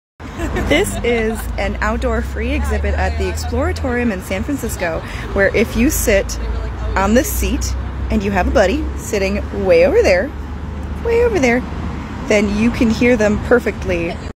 Science at San Francisco Exploratorium || ViralHog
Occurred on October 3, 2021 / San Francisco, California, USA

"My brother, my friend, and I went to San Francisco one October weekend to visit the Exploratorium. Outside that building is a free exhibit showing the scientific concept of parabolic shapes and sound waves. With one person on each chair, they can perfectly converse with each other at normal talking volume despite being super far away. SCIENCE!"